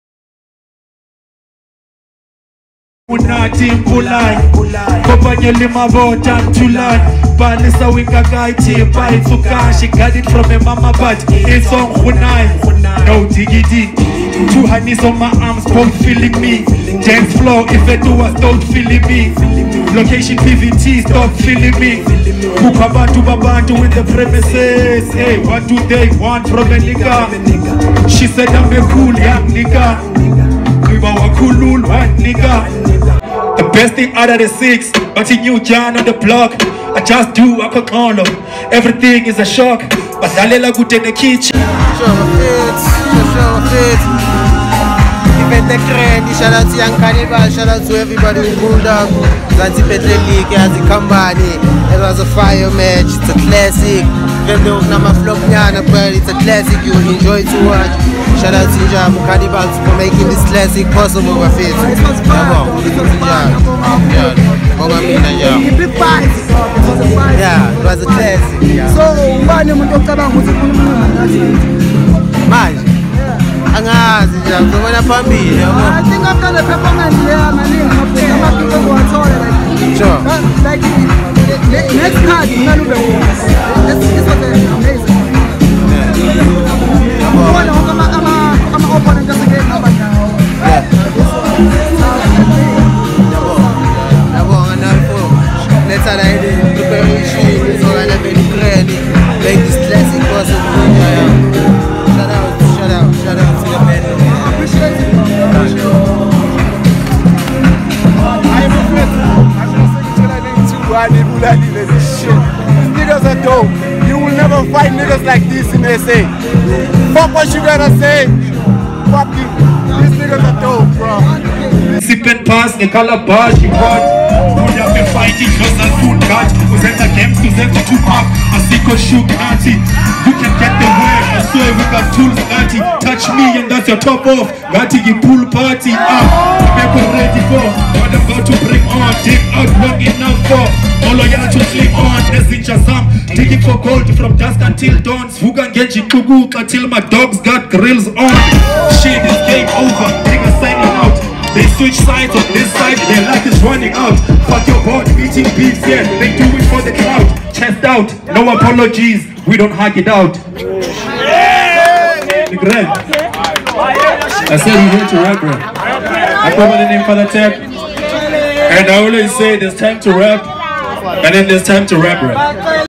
SLOW JAM MASHESHA Recaping his Battle with YOUNG CANNIBAL
SLOW JAM MASHESHA vs YOUNG CANNIBAL | eGaliniTV